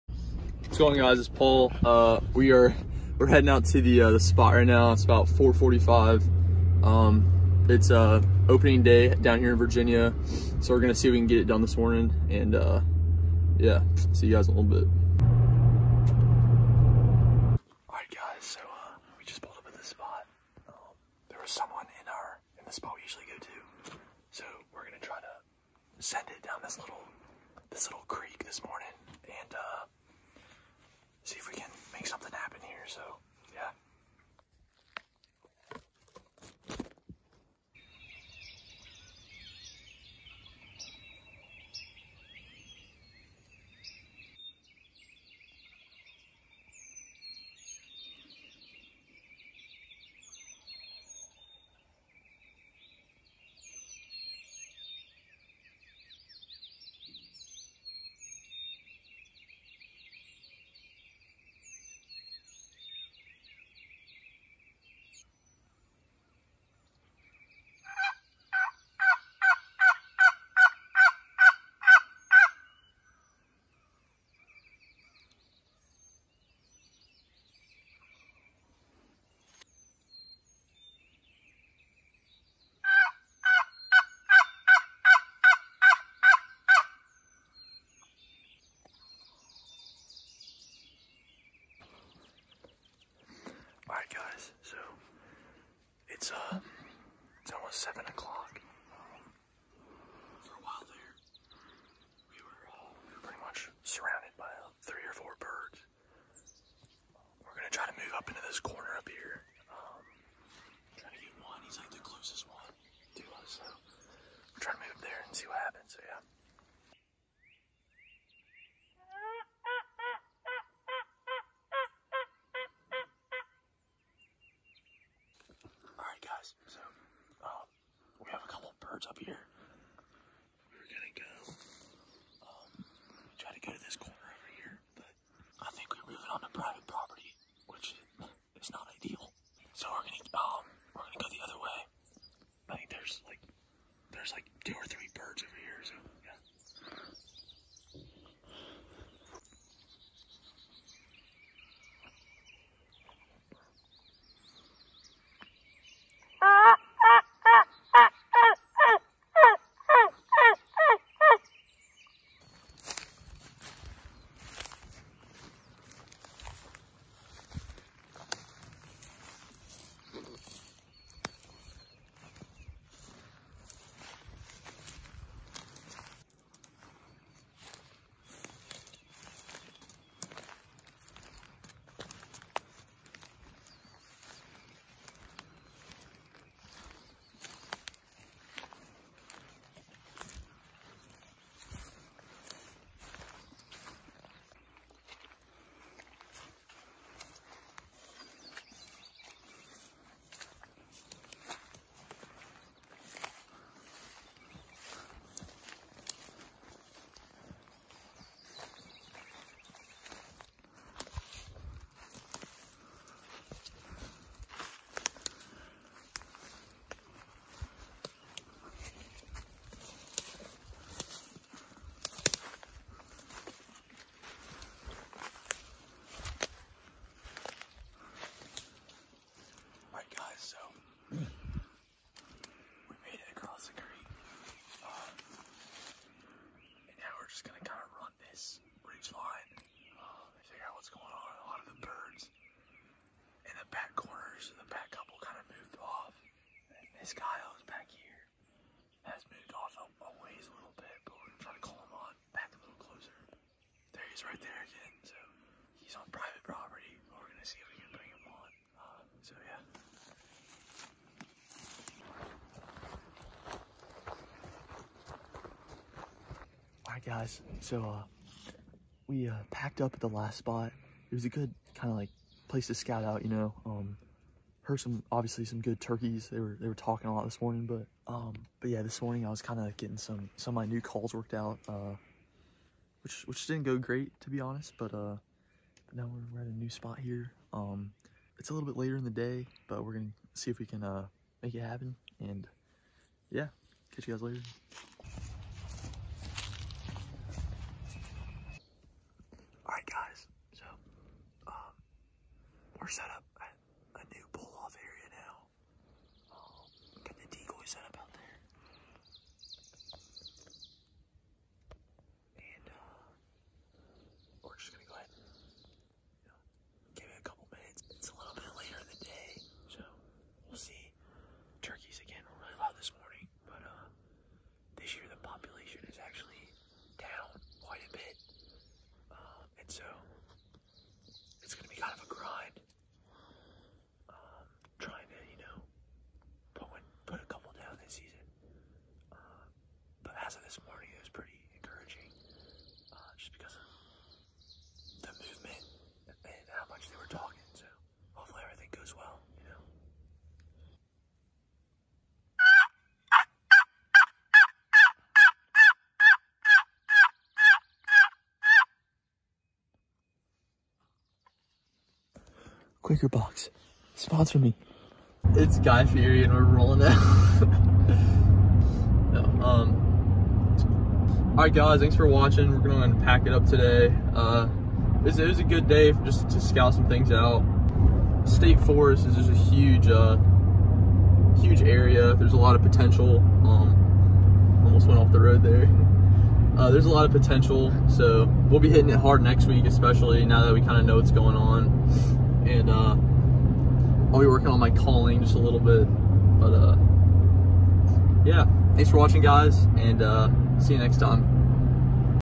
OPENING DAY/ Virginia Turkey Hunting Pt. 1
Hey Ya'll! Its been a while! For the next couple of weeks, Sam and I will be filming and documenting our season. Enjoy part 1, stay safe!